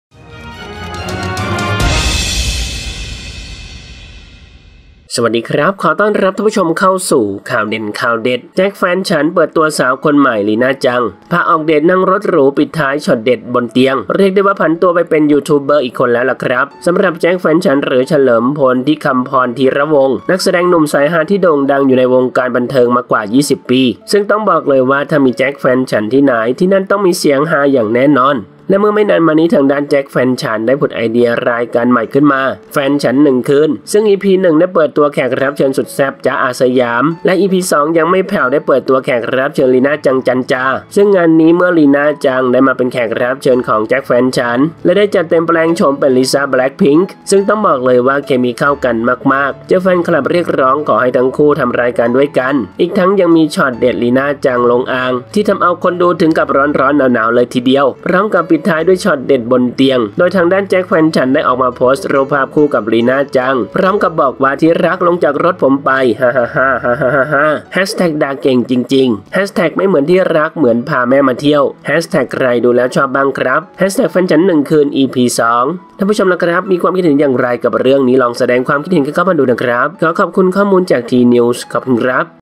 แจ็ค แฟนฉัน เปิดตัวสาวคนใหม่ ลีน่าจัง พาออกเดทนั่งรถหรู
ขอบคุณ Jackfanchan